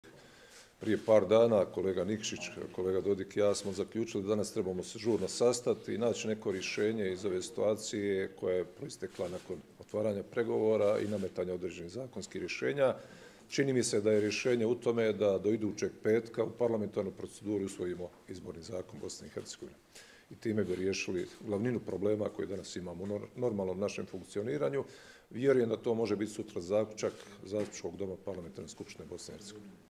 DRAGAN ČOVIĆ: Pitanje izbornog zakona riješiti u parlamentu
Предсједник ХДЗ-а БиХ Драган Човић рекао је да би проблеми у БиХ били ријешени усвајањем изборног закона БиХ у Парламентарној скупштини БиХ.